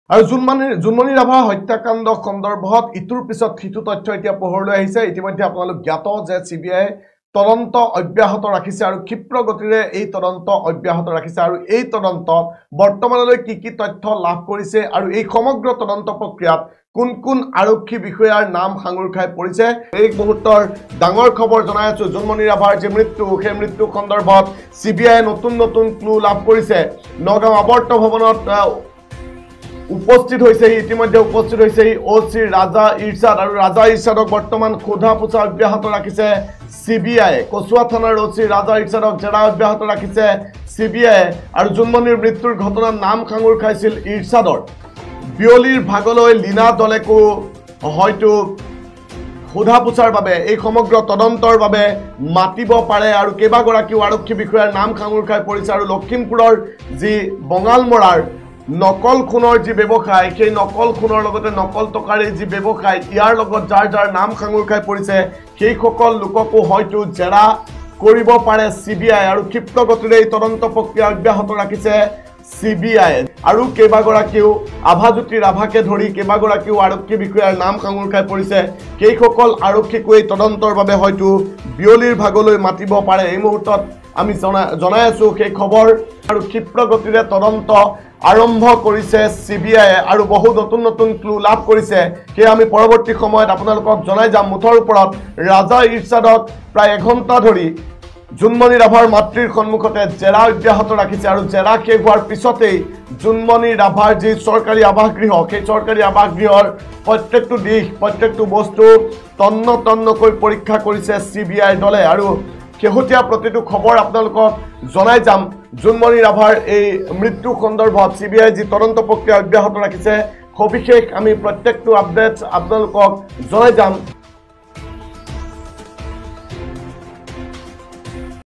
অৱশেষত, জোনমণিৰ মৃত্যুৰ বিস্ফোৰক তথ্য CBIৰ হাতত। কোন কোন আৰক্ষী বিষয়া পৰিব জালত।
This video is only for the information